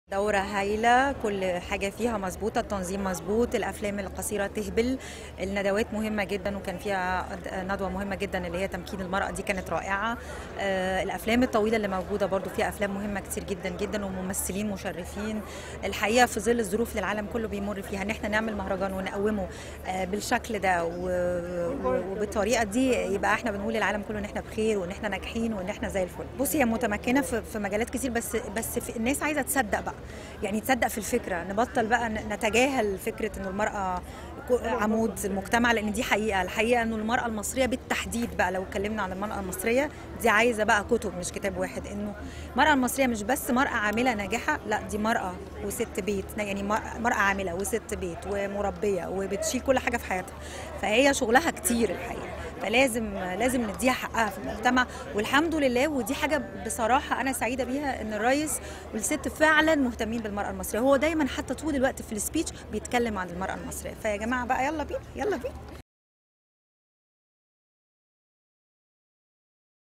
لقاء الخميسي : " ندوة تمكين المرأة كانت إضافة عظيمة من مهرجان الجونة السينمائي"
تقرير: مني الموجي
تصوير: نسيم عبدالفتاح
مونتاج : محمد قطب